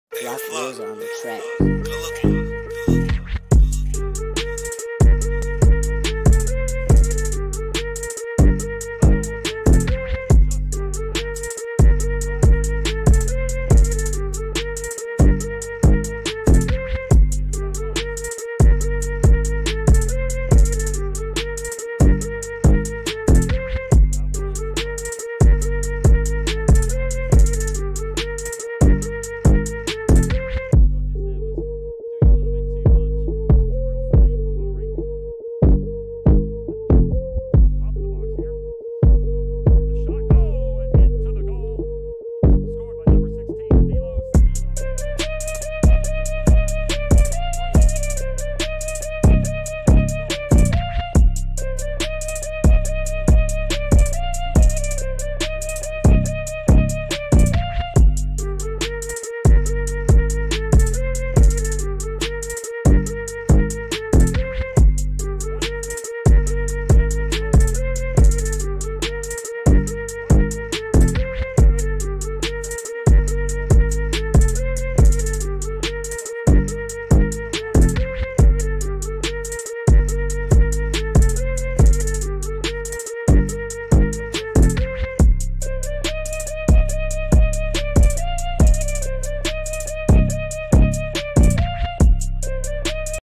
Highlights at Appalachian FC 5/7/2021
Danilo Seglio scored a brace and Aidan O'Brien scored just before halftime to secure a 3-1 win for the Otters in the first leg of the OtterSquatch Cup!